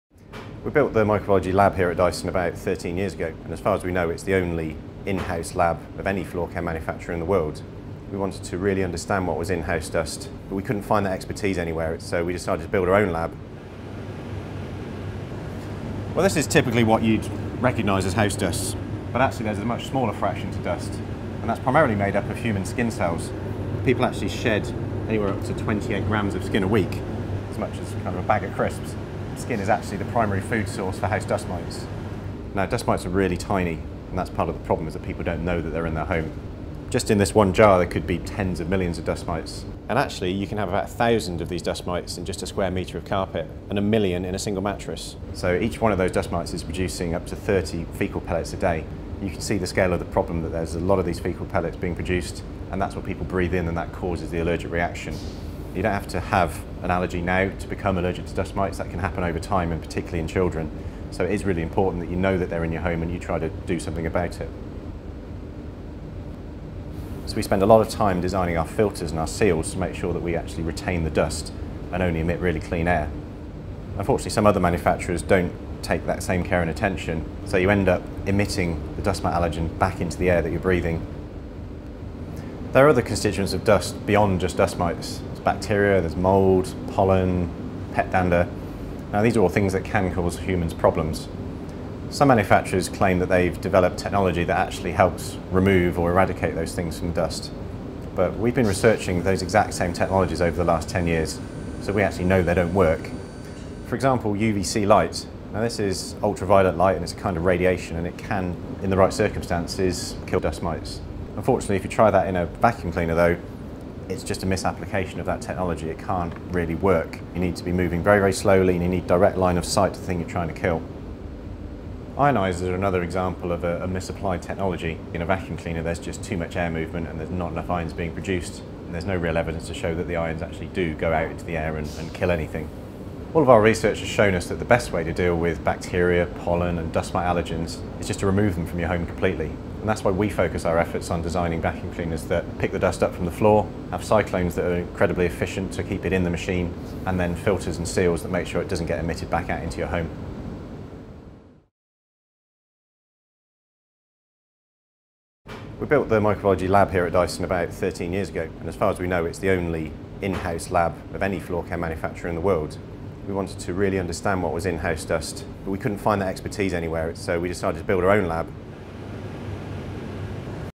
Down in the dust lab: Dyson microbiologists tackle dust mites, dirt and allergens
Down in the dust lab: Dyson microbiologists tackle dust mites, dirt and allergens , Down in the dust lab: Dyson microbiologists tackle dust mites, dirt and allergens